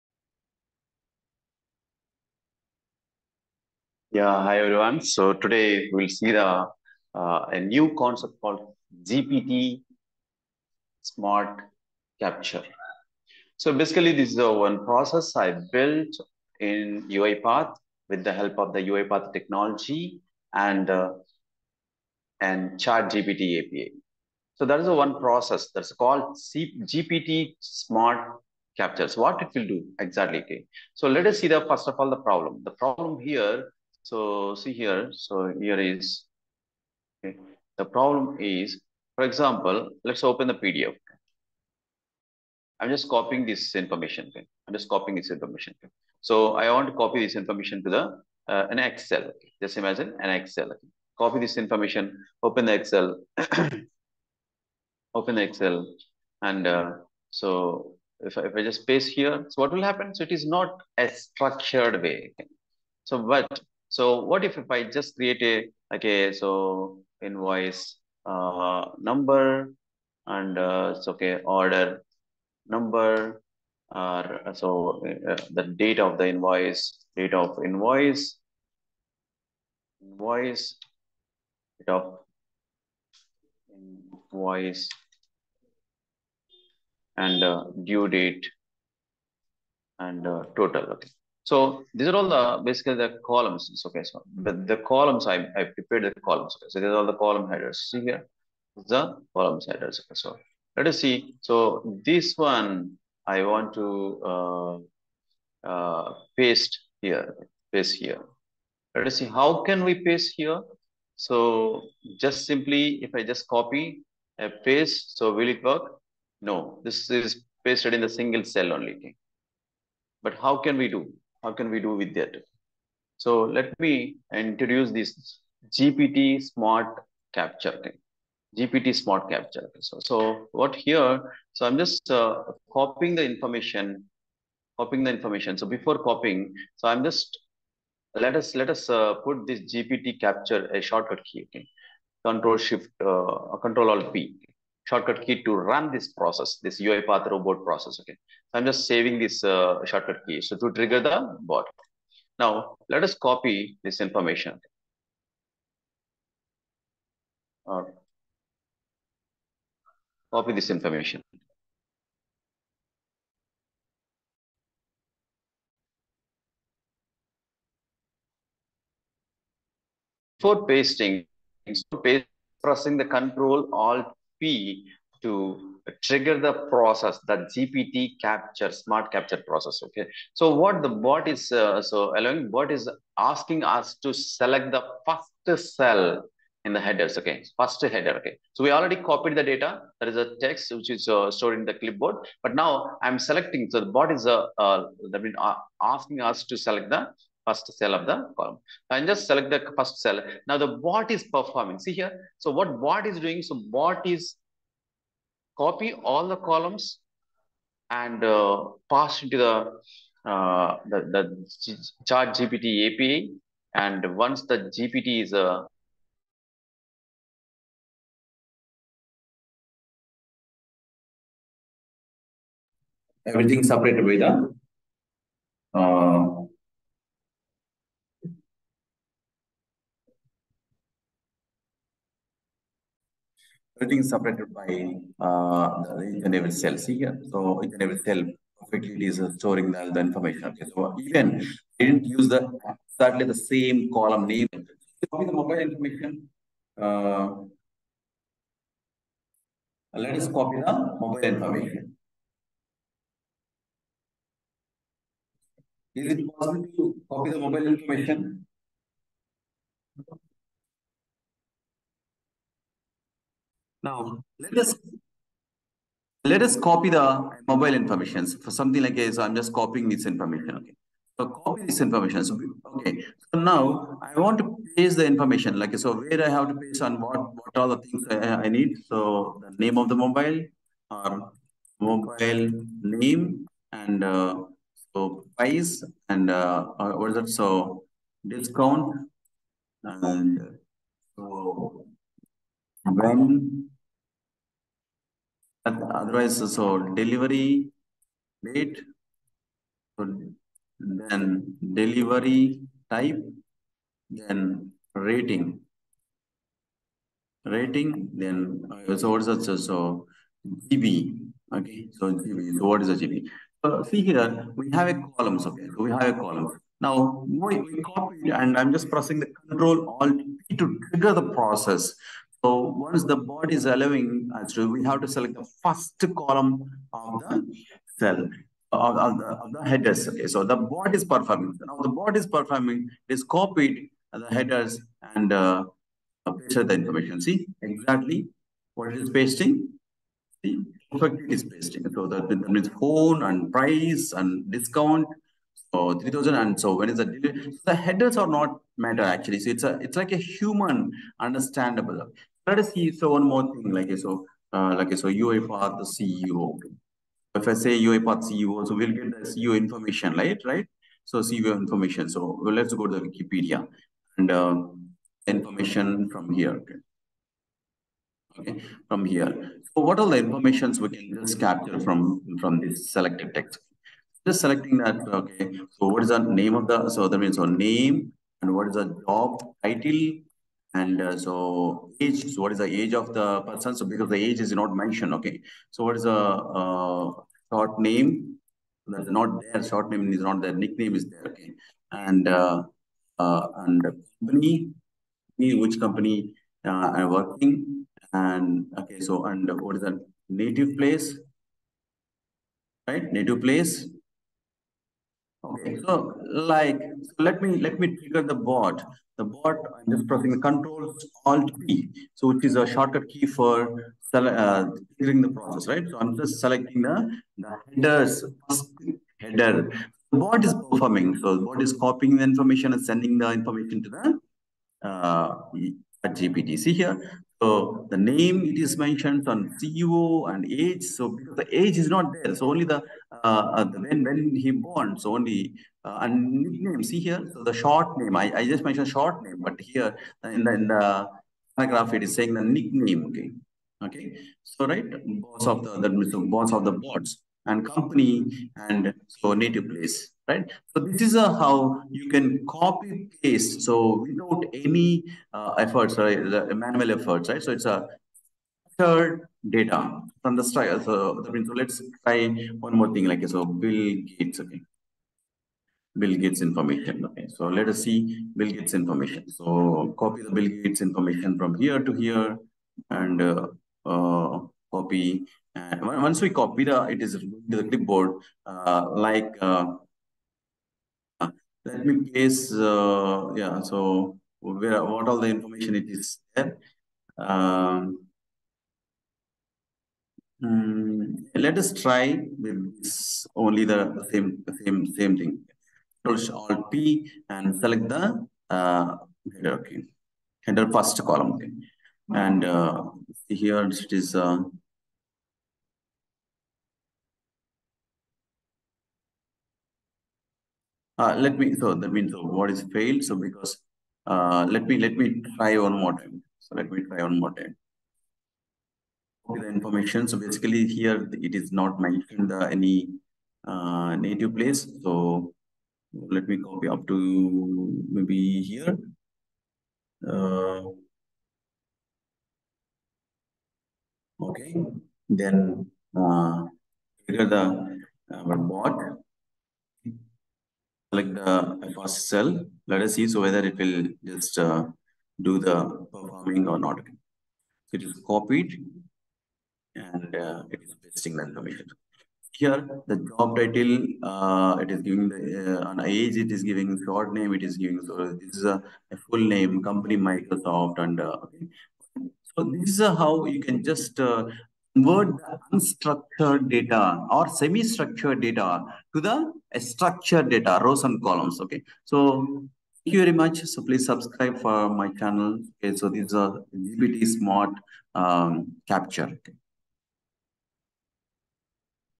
How to Transform Unstructured Data into Structured Data with GPT-Smart Capture | UiPath | BotsDNA
GPT-Smart Capture is a robust tool that enables the conversion of unstructured data into structured data within UiPath. The tool streamlines the data extraction process by eliminating the need for manual data entry. The workflow begins with a human copying unstructured data from any source, such as a PDF, web page, text document, or application. Next, the human specifies the desired data fields by opening Excel and entering them. The bot is triggered by a shortcut key, prompting the user to select the first cell of the desired field in Excel. The bot then collects all the data fields, including headers, and sends the information, along with the unstructured data, to Chat GPT API from UiPath. The Chat GPT API returns the values required, and UiPath parses the data and inserts it back into the corresponding Excel cells. By automating the data extraction process with GPT-Smart Capture, data extraction is more efficient and accurate, freeing up valuable time for other critical tasks.